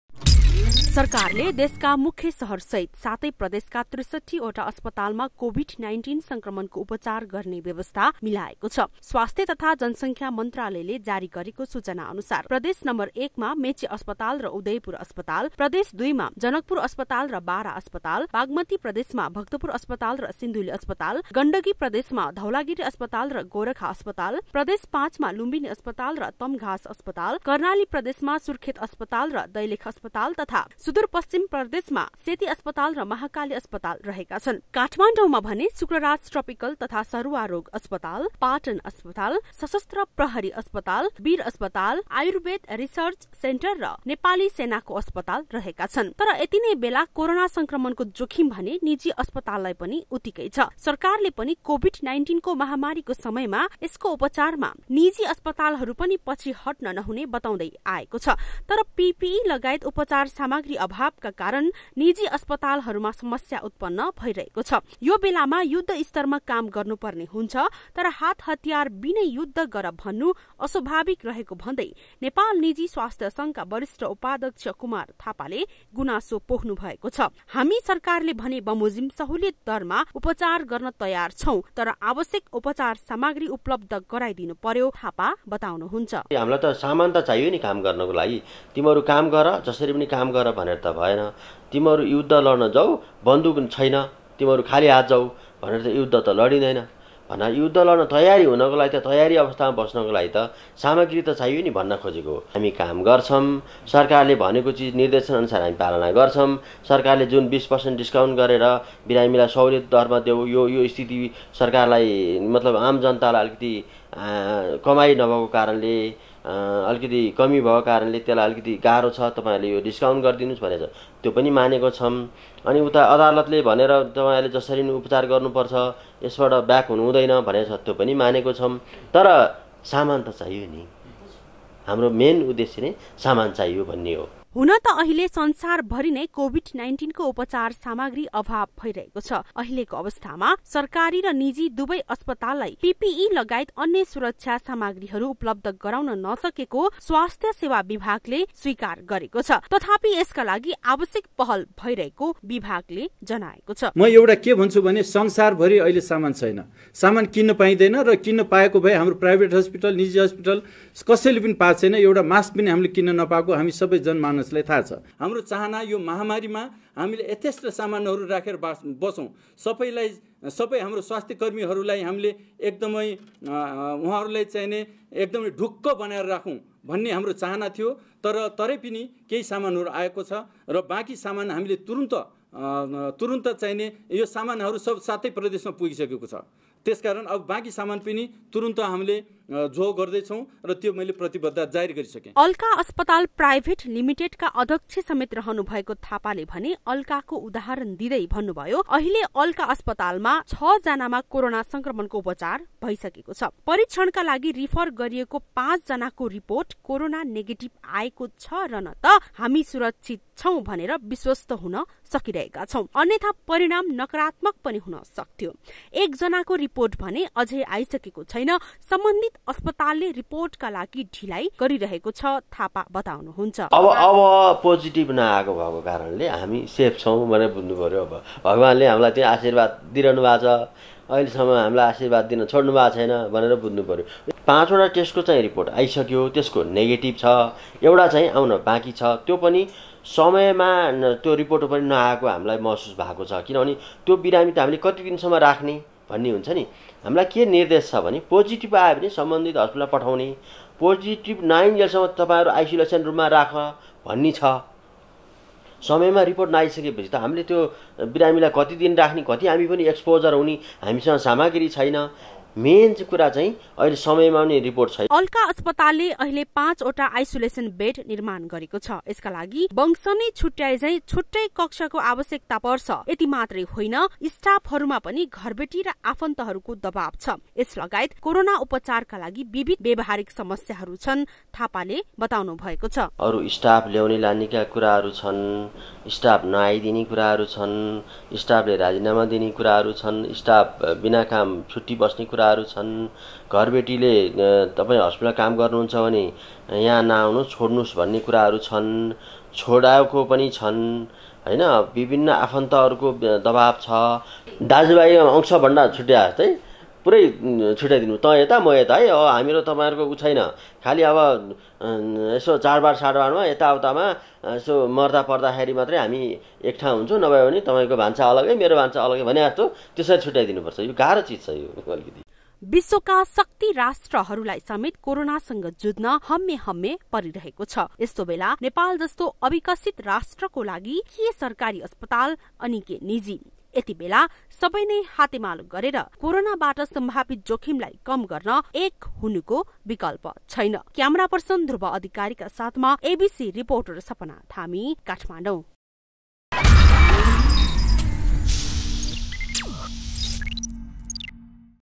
ABC REPORT || निजी अस्पताल
देशमा अहिले कोभिड – १९ को सम्भावित जोखिमलाई सामना गर्न विशेषगरी सरकारी तथा निजी अस्पतालले के गर्न सक्छन् भन्ने विषयमा बहस एवम् तयारी समेत भईरहेको छ । पीपीई लगायतका उपचार सामाग्रीहरुको अभावका कारण निजी अस्पतालहरुमा पनि समस्या उत्पन्न भईरहेको छ । यी सामाग्रीहरुको उपलब्धताका लागि सरकार लगायत सम्बन्धित निकायबाट आवश्यक पहल भएमा सरकारको निर्देशन पालना गर्न तयार रहेको निजी अस्पतालहरुको भनाई छ ।